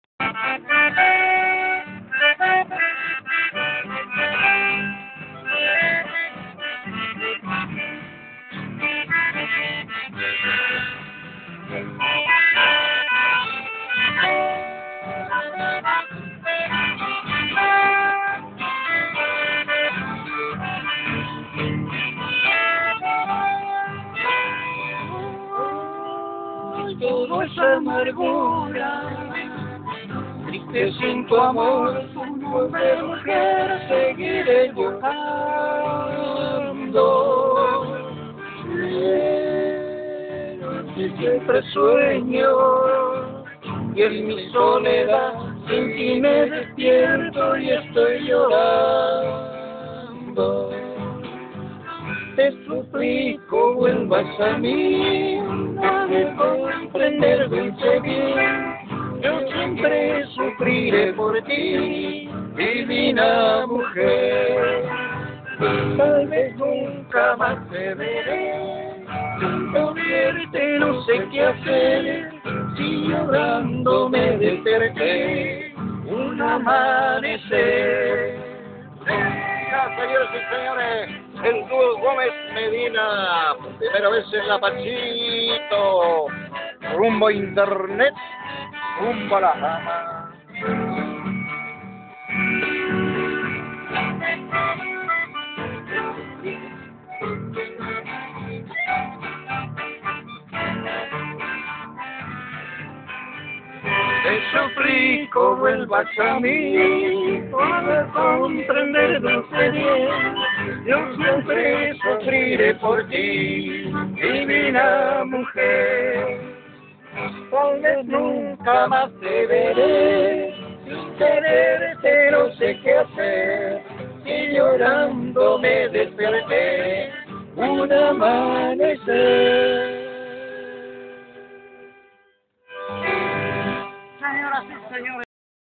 cesario almiron con duo gomez medina III.3gp
Ensayo de Gomez Jose Aurelio en Lapachito en la casa de Cesario Almiron para la presentacion en Laguna Limpia